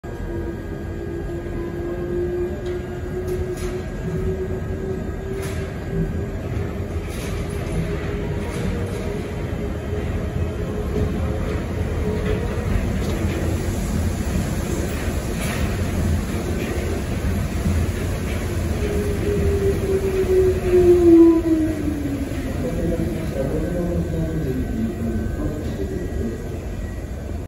[일본철도] 230120 사이타마 뉴 셔틀 이나선 오미야행 열차 오미야역 도착 | 埼玉ニューシャトル大宮駅到着
150여개의 글이 있는 블로그와 150여명의 회원이 가입되어 있는 오픈채팅방에 방문해보세요!

* 영상 사용 시, 출처와 댓글을 꼭 남겨주시면 감사하겠습니다.